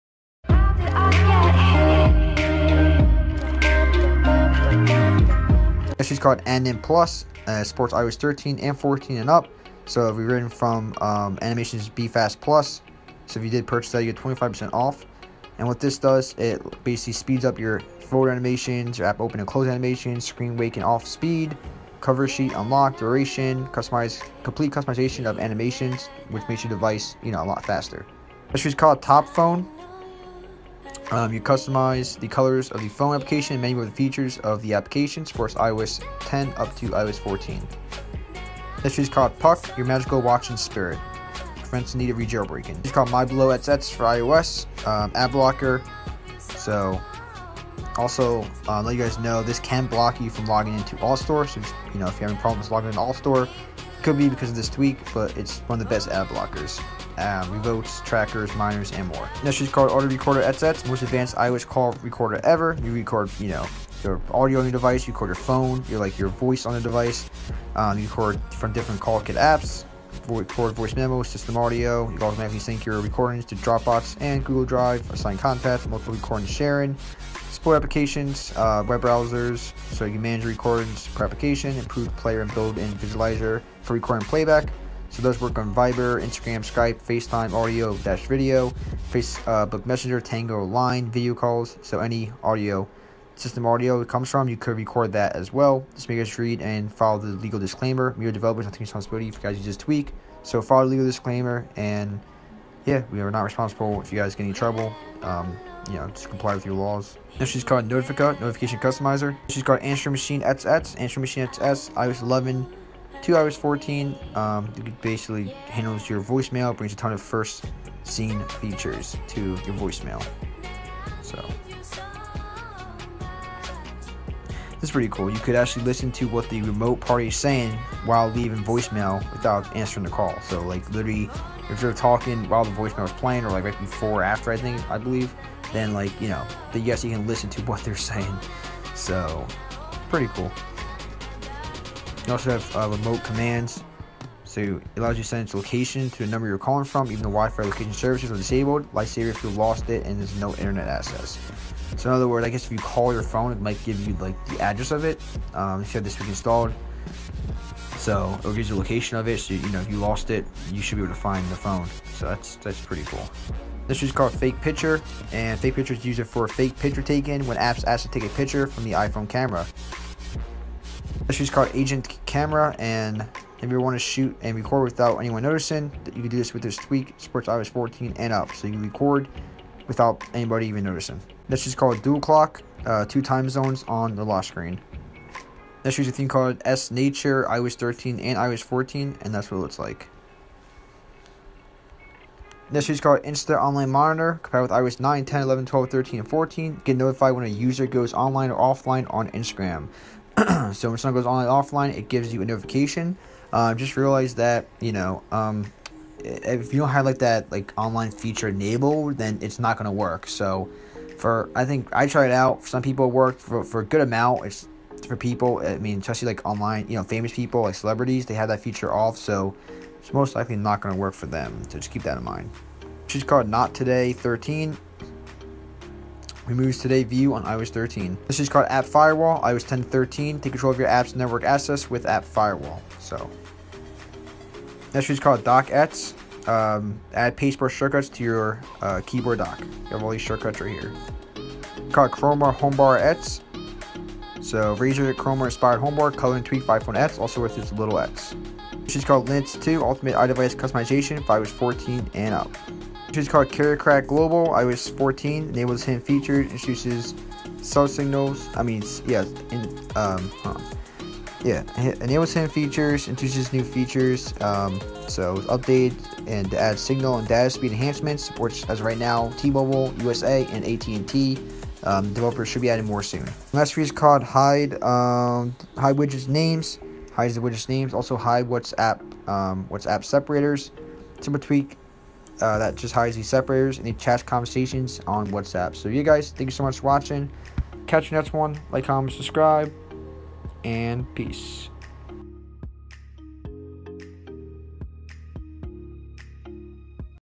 Top BEST Updated Jailbreak Tweaks for iOS 11/12/13/14 (Checkra1n, Odyssey, Unc0ver + Chimera)
In this video I show you my top & best updated iOS 11 / 12 / 13 / 14 jailbreak tweaks compatible with the Checkra1n, Odyssey, Chimera and Unc0ver jailbreaks. Comment down any questions or concerns you may have. Peace!